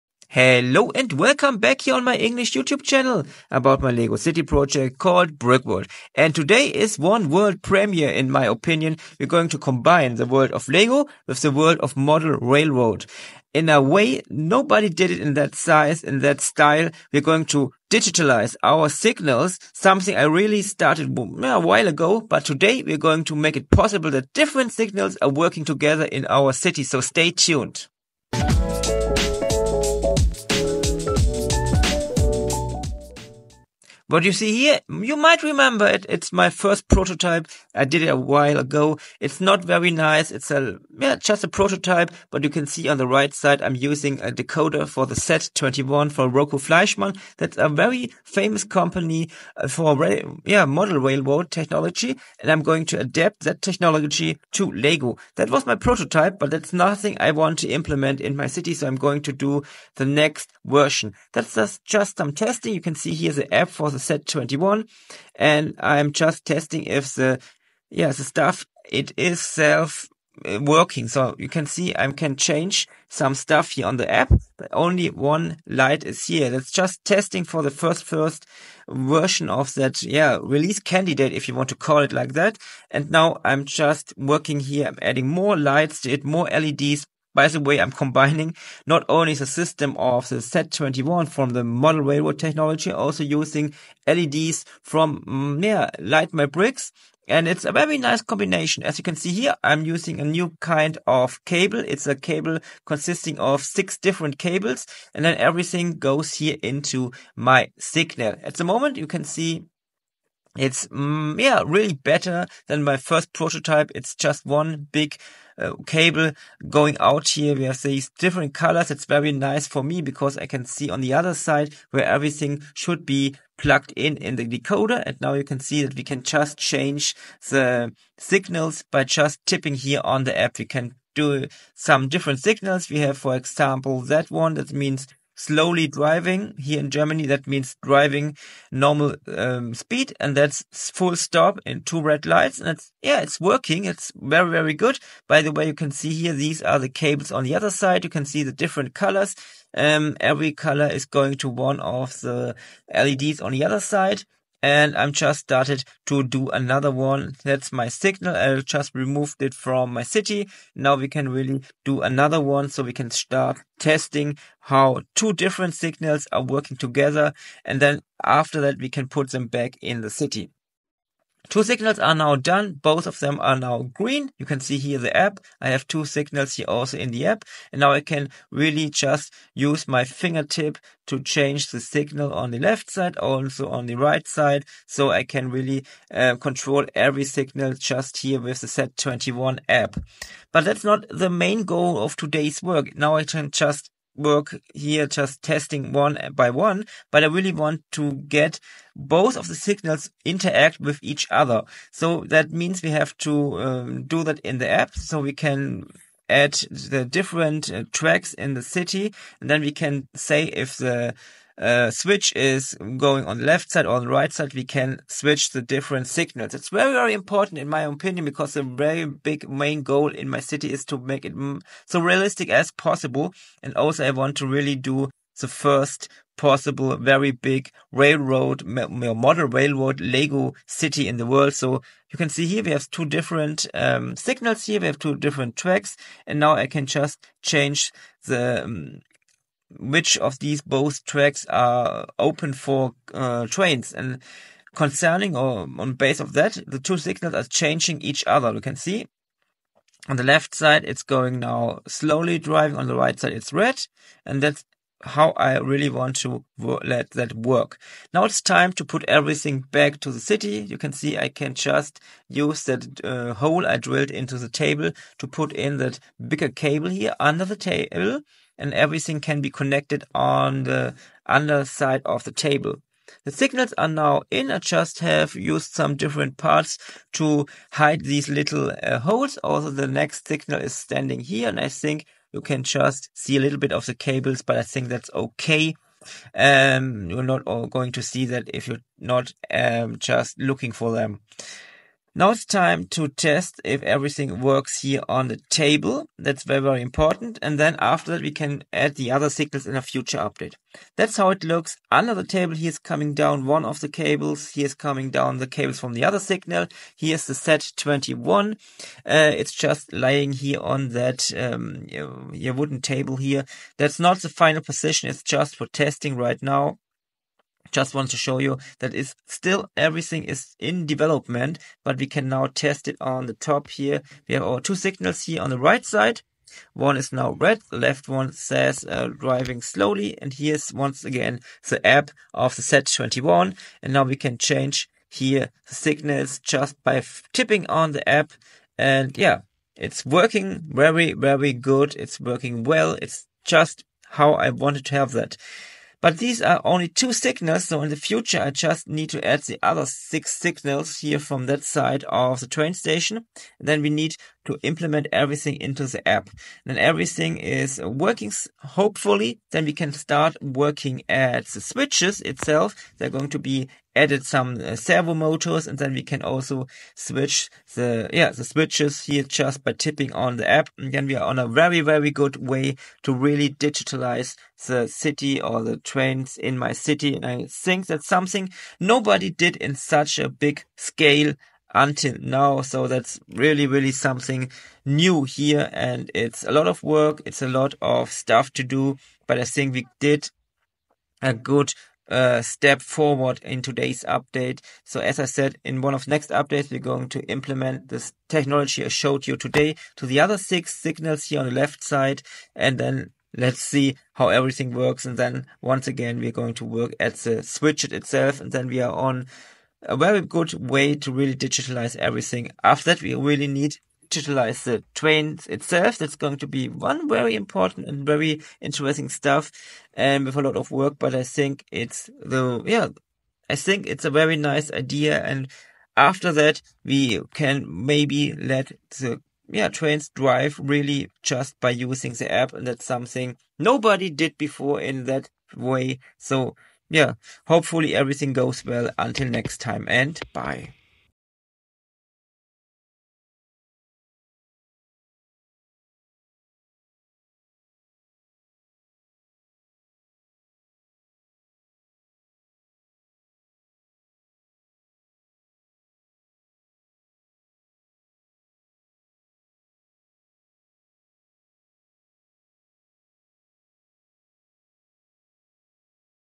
World Premiere? Functional LEGO® Train Signals Now Interacting!
Today marks a significant leap in the digitalization of railroads in the Brick World as we unveil functional train signals that interact with each other! This might just be the first time LEGO® and model railroading technology are combined in such a way and on this scale. Join us for this exciting world premiere!

▬ Social Media ▬▬▬▬▬▬▬▬▬▬▬▬▬▬▬

LEGO® is a trademark of the LEGO Group. This channel is not sponsored, authorized or endorsed by the LEGO Group.